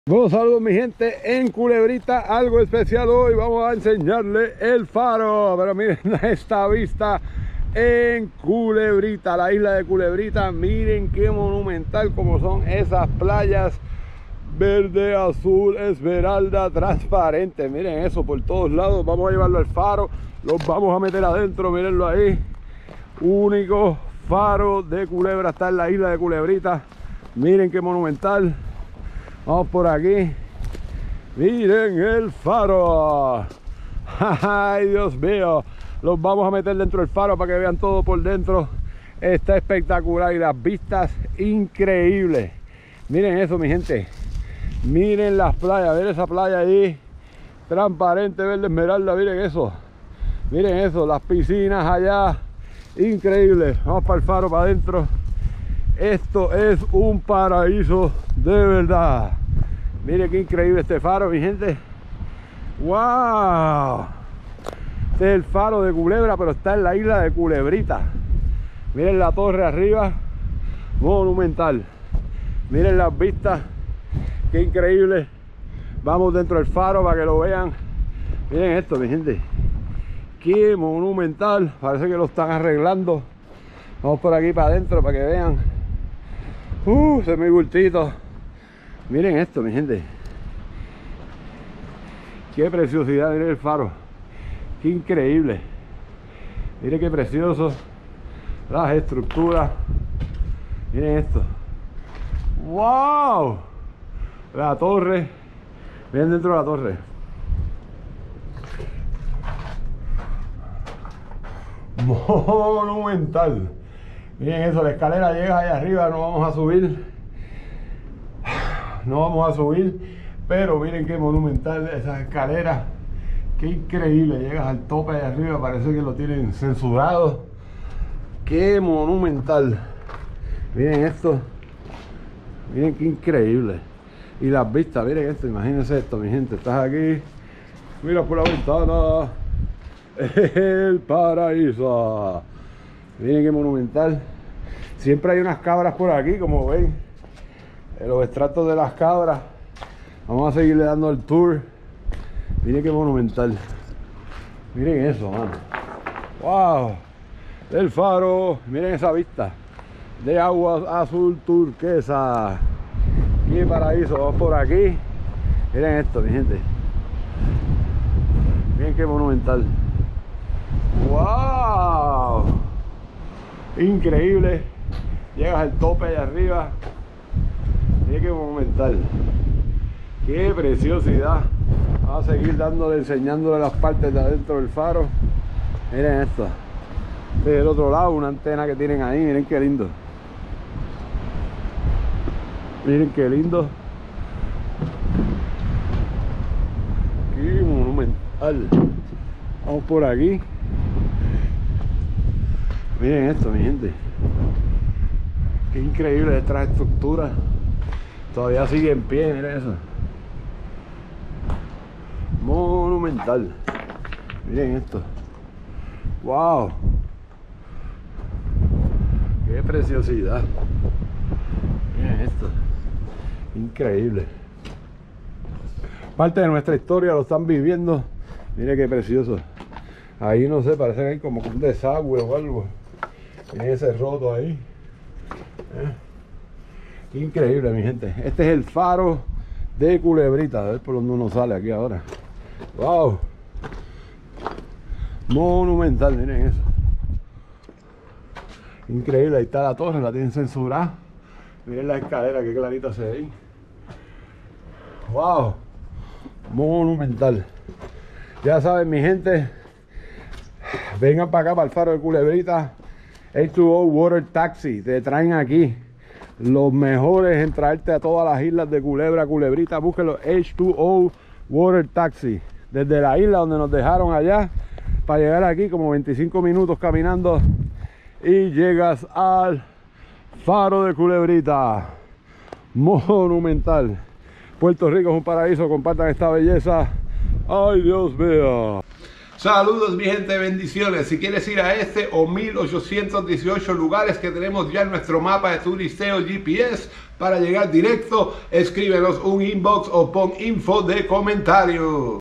el FARO de CULEBRITA 🕌🇵🇷 y Te Metemos Adentro 😍 con las Vistas Mas Monumentales de la Isla !!
el FARO de CULEBRITA 🕌🇵🇷 y Te Metemos Adentro 😍 en Tope de Montaña con las Vistas Mas Monumentales de la Isla !!